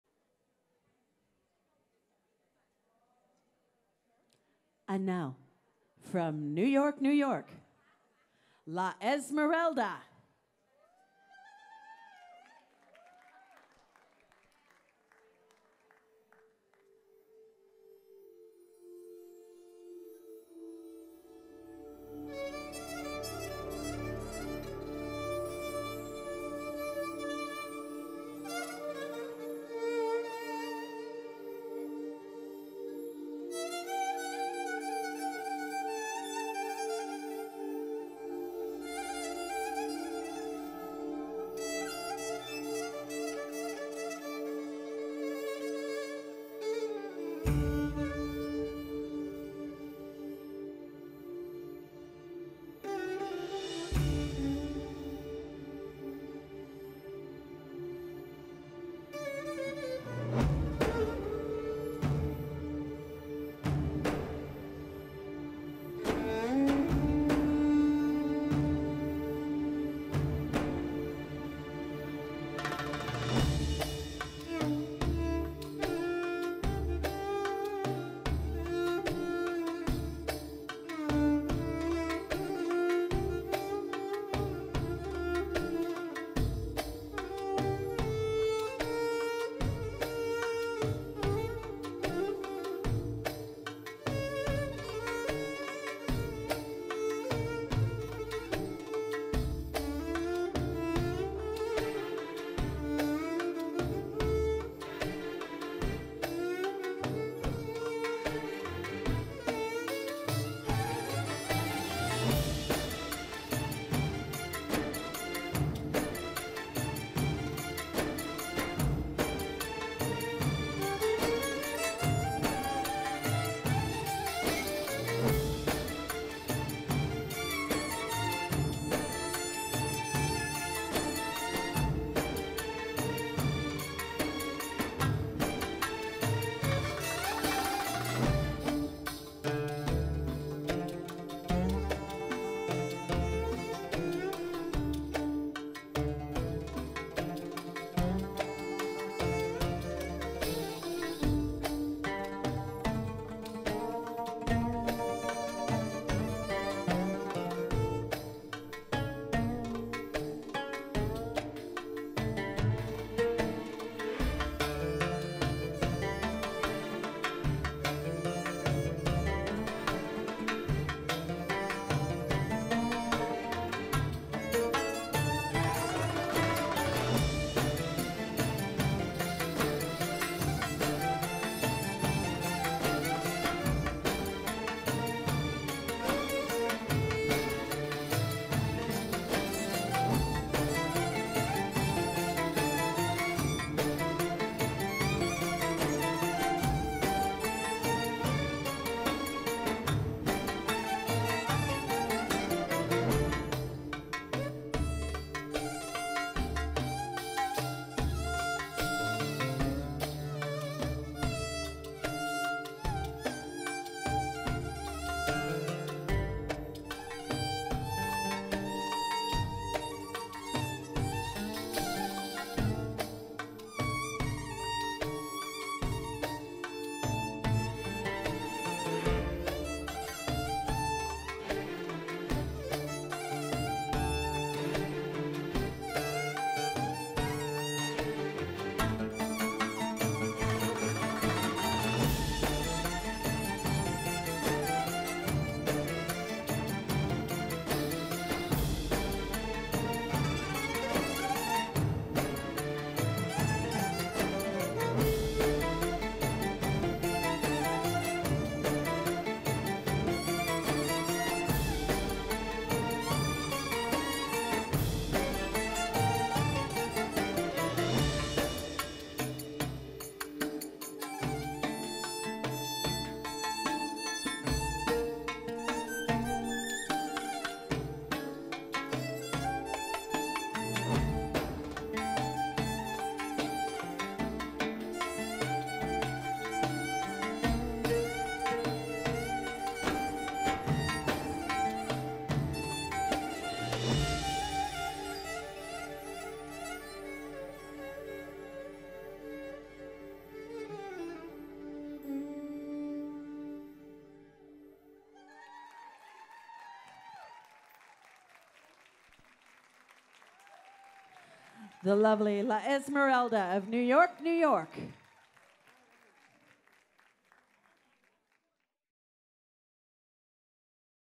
Yearning
I feel an intense yearning for love… to be again, to express my real feelings, to experience myself in freedom “not debauchery.” Without receiving offenses… Without doubting of my womanhood or being questioned…

19th Annual Rakkasah East